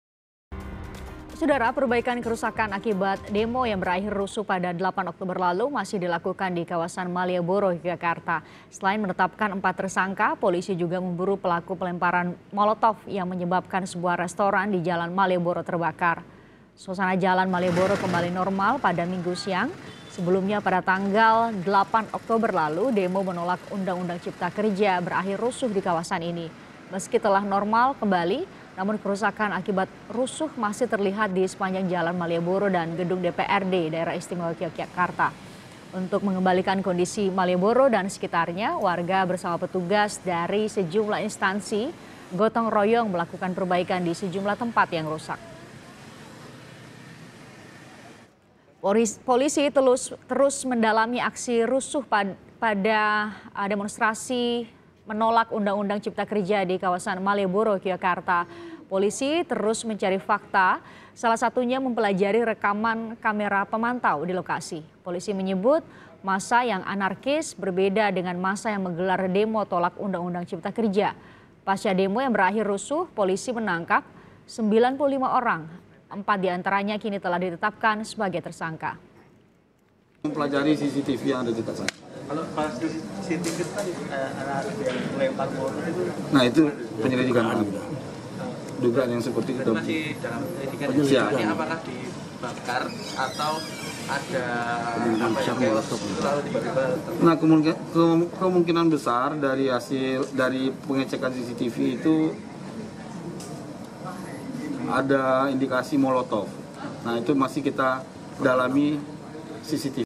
Begini Penampakan Kerusakan Pasca Demo Tolak UU Cipta Kerja di Malioboro
YOGYAKARTA, KOMPAS.TV - Suasana Jalan Malioboro kembali normal pada Sabtu (10/10/2020) siang.

Sebelumnya pada tanggal 8 Oktober lalu, demo menolak Undang-undang Cipta Kerja berakhir rusuh di kawasan ini.

Meski telah normal kembali, namun kerusakan akibat rusuh masih terlihat di sepanjang Jalan Malioboro dan Gedung DPRD DIY.

Untuk mengembalikan kondisi Malioboro dan sekitarnya, warga bersama petugas dari sejumlah instansi gotong royong melakukan perbaikan di sejumlah tempat yang rusak.

Polisi terus mendalami aksi rusuh pada demo tolak Undang-undang Cipta Kerja di kawasan Malioboro Yogyakarta.

Polisi terus mencari fakta, salah satunya mempelajari rekaman Cctv di lokasi.

Polisi menyebut massa yang anarkis berbeda dengan massa yang menggelar demo tolak Undang-undang Cipta Kerja.

Pasca demo yang berakhir rusuh, polisi menangkap 95 orang dimana 4 diantaranya kini telah ditetapkan sebagai tersangka.